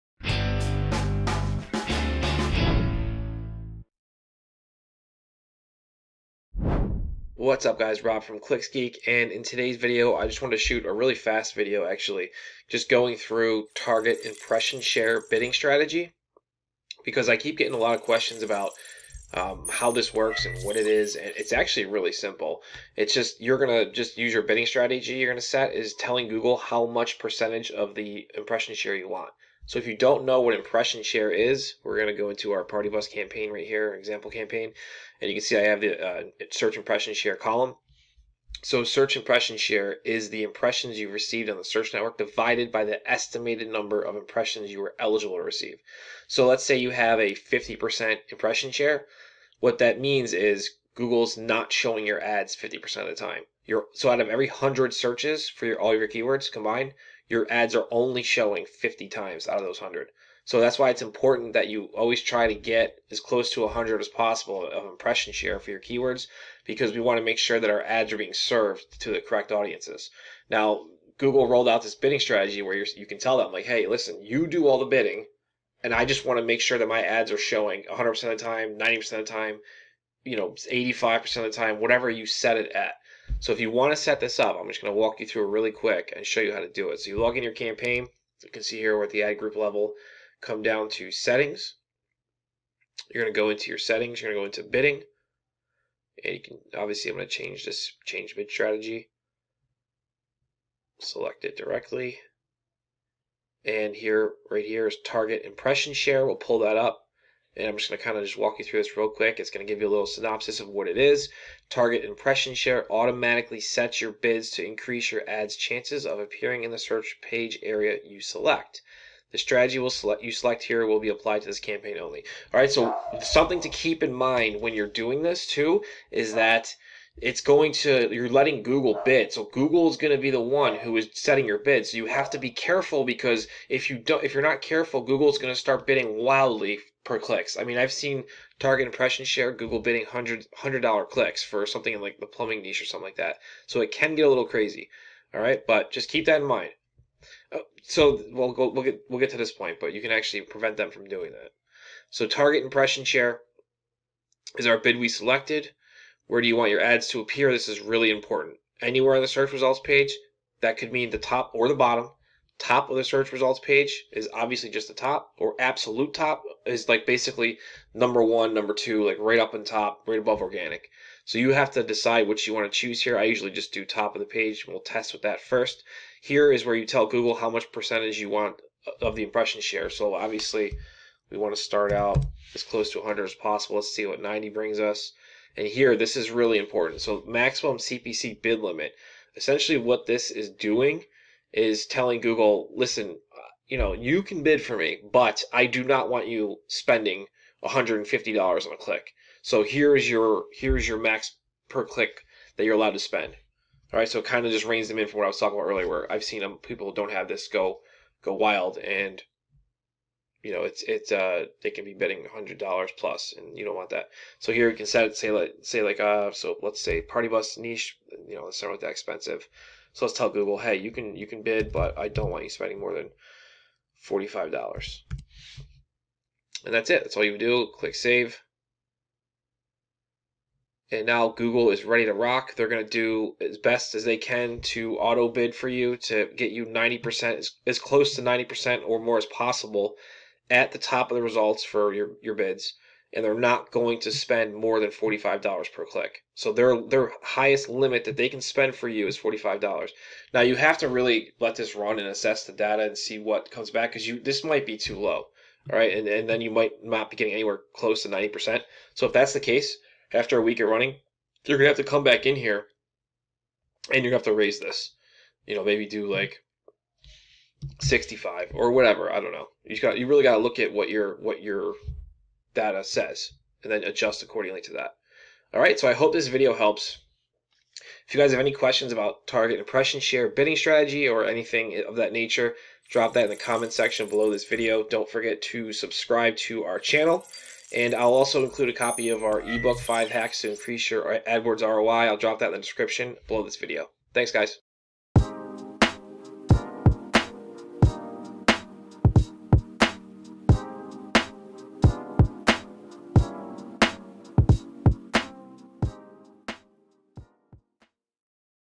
Target Impression Share Bidding EXPLAINED | Full Bid Strategy Walk-Through
How to use Target Impression Share bidding strategy EFFECTIVELY. When used correctly, Target Impression Share can be a juggernaut of lead generation for your Google Ads campaigns. BUT, if it's not used properly it could tank your campaign. REALLY BAD. Inside this video I walk you through how it works and the best practices of how and when to use it on your campaigns. Also, what the one thing is that you MUST implement when deciding to run this bidding strategy. Enjoy!

⏱️TIMESTAMPS⏱️
0:00 Intro
0:32 What is search impression share?
1:27 How to setup target impression share in your campaign
5:00 Outro